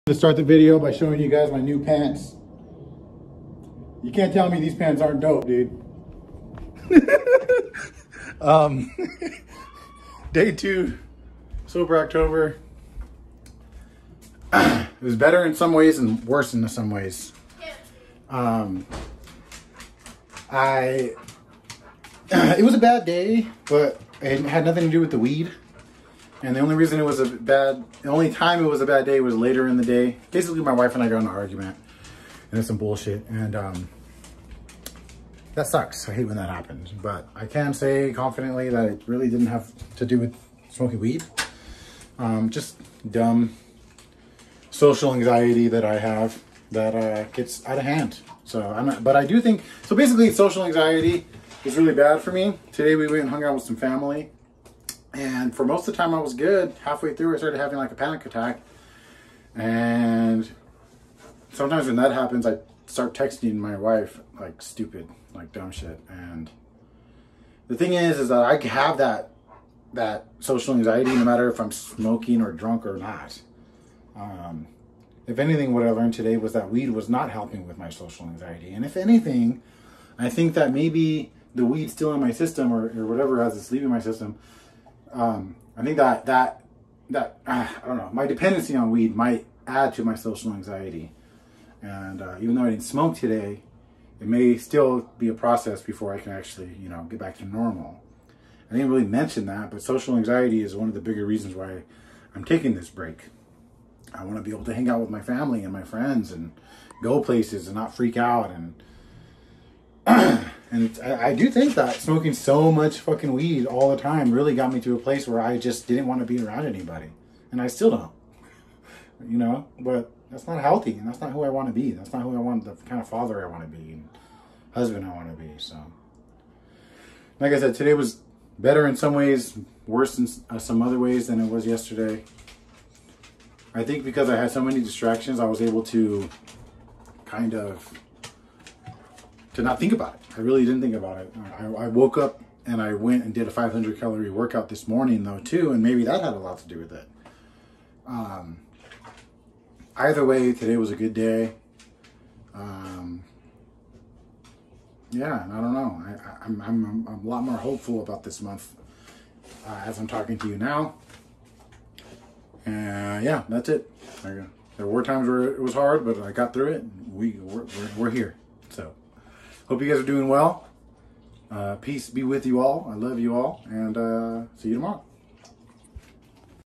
DAY 2 with NO WEED! 😅 | SOBER OCTOBER 2022
Today wasn’t so bad…kinda. I think the hard workout in the morning and the distraction of being with family helped a lot. Love you all and see you tomorrow! 😎